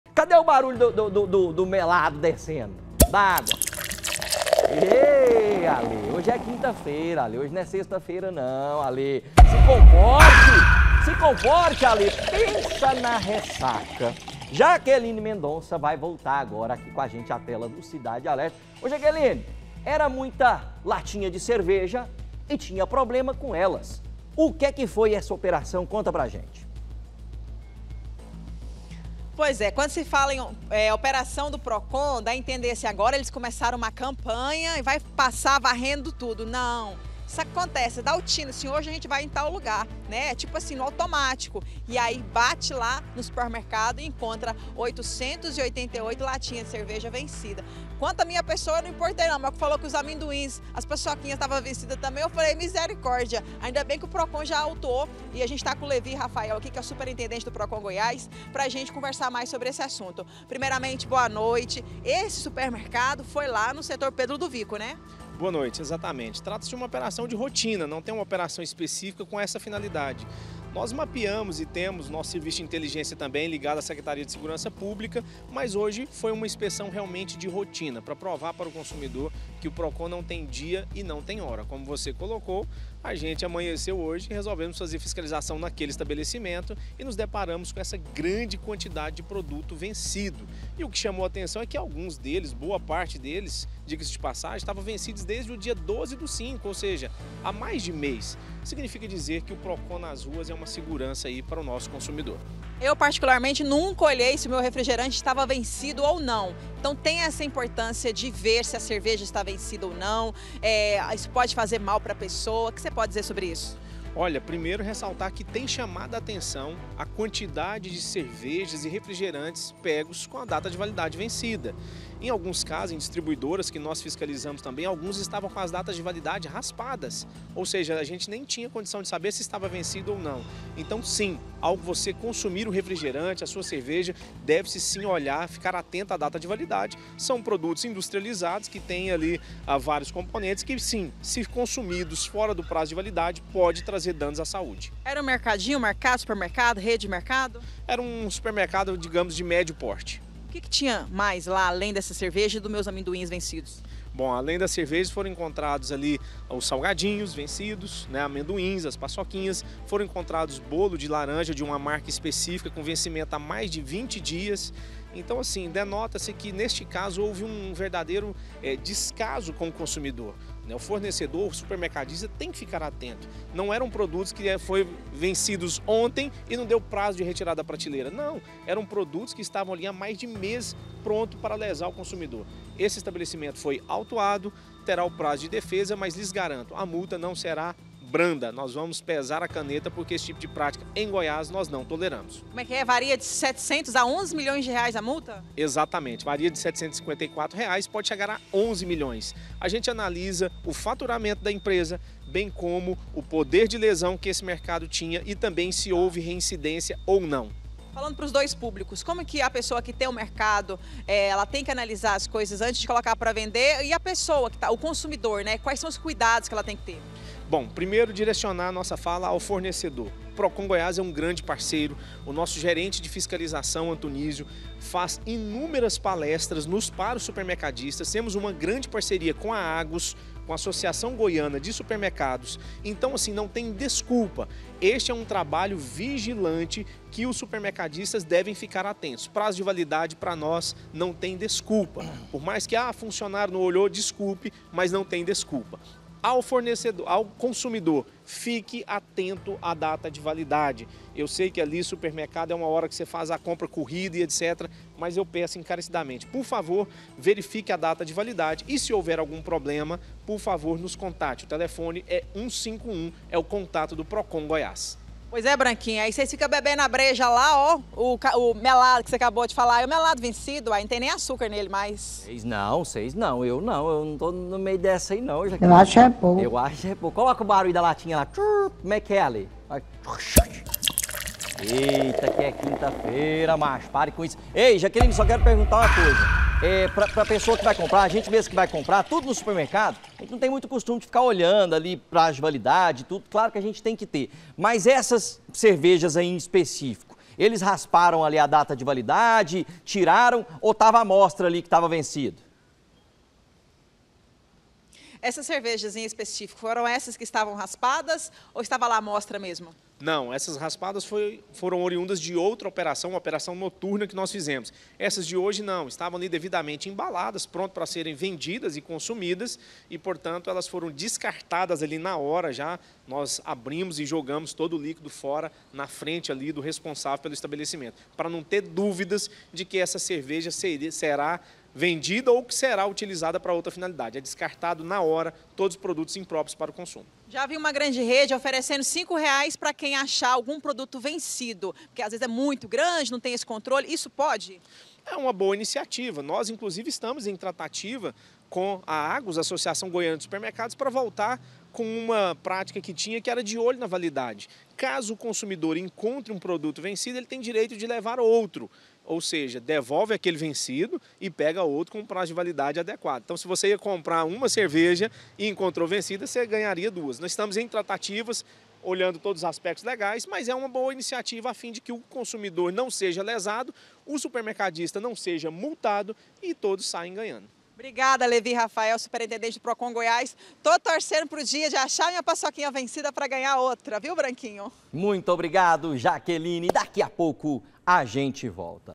PENSA NA RESSACA: MUITA CERVEJA VENCIDA APREENDIDA EM SUPERMERCADO
PROCON GOIÁS APREENDE MAIS DE 800 LATINHAS DE CEVERJA VENCIDA EM SUPERMERCADO.